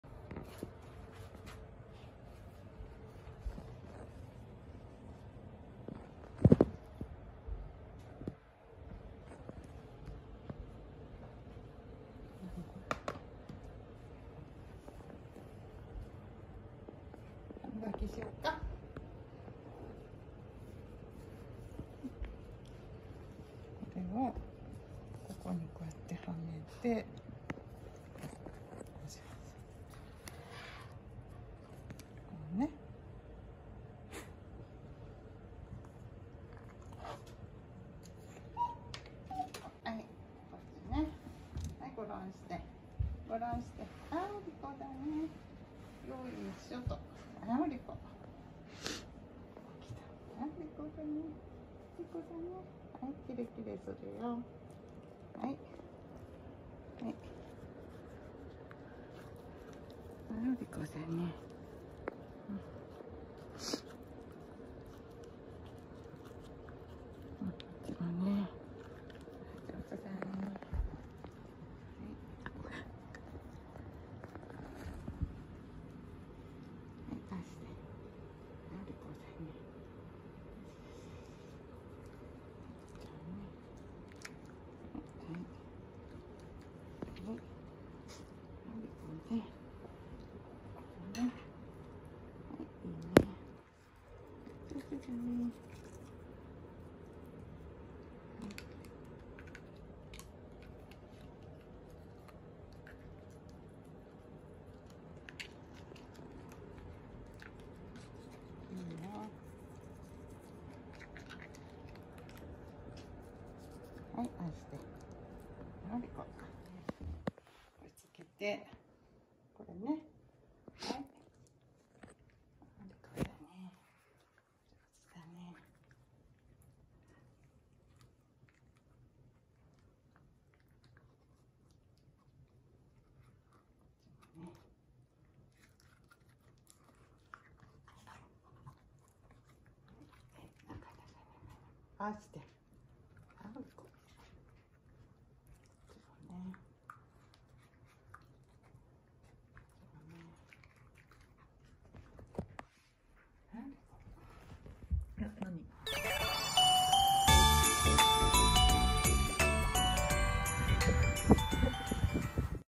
韓国発オーガニックコットンの指歯ブラシで歯磨き体験をする秋田犬姉妹【 Akita-inu】
韓国発、ラフィングチャーリーさんの指歯ブラシはオーガニックコットンなので安心安全👍ただ、手の小さい私には大き過ぎました😅中で少し縫って使おうと思います！

虎毛秋田犬ぱたこ５歳
赤毛秋田犬こむぎ４歳

Patako:Brindle girl
              5 years
Komugi:Red girl
              4 years